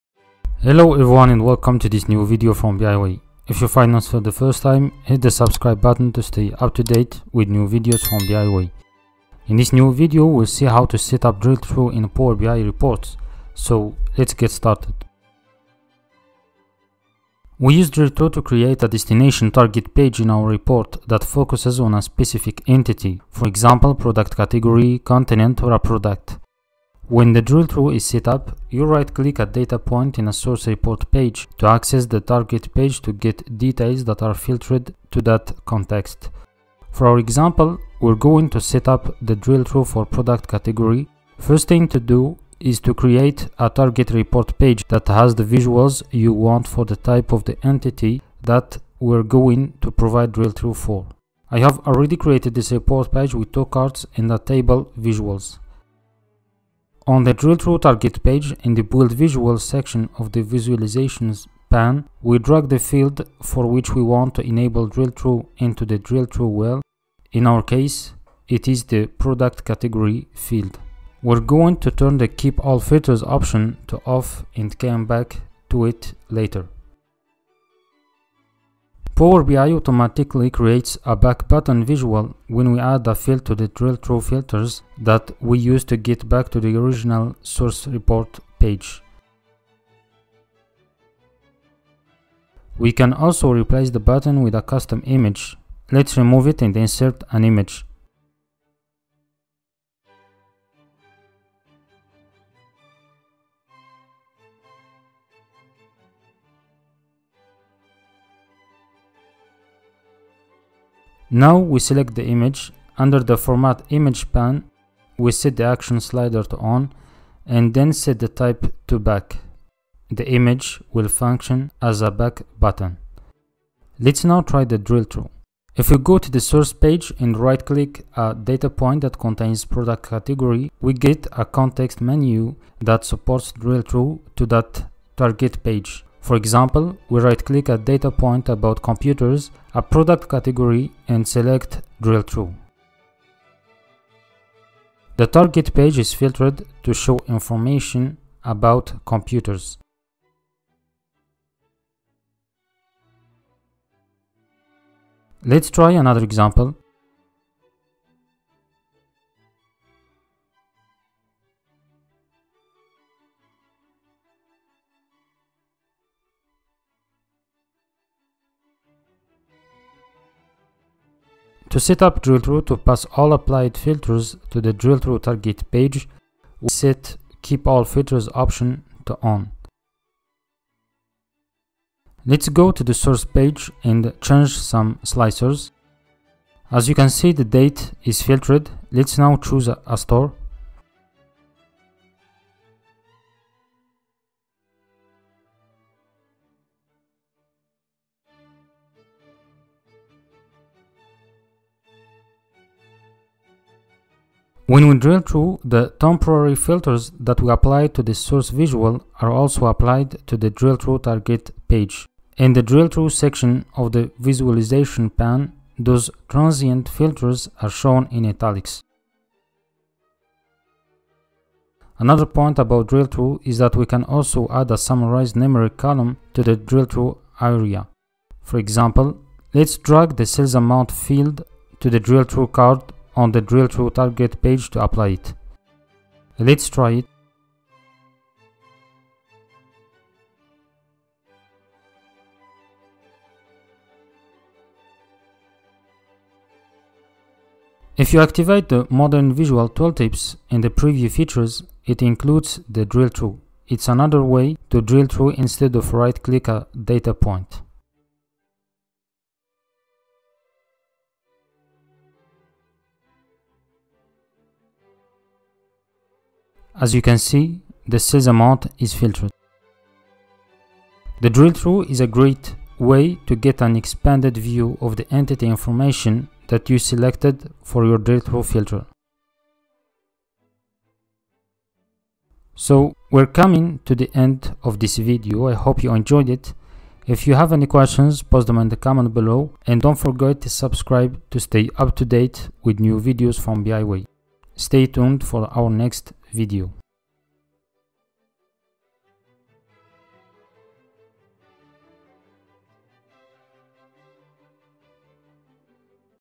Drillthrough in Power BI
Set up drillthrough in Power BI
Use drillthrough to create a destination target page in your report that focuses on a specific entity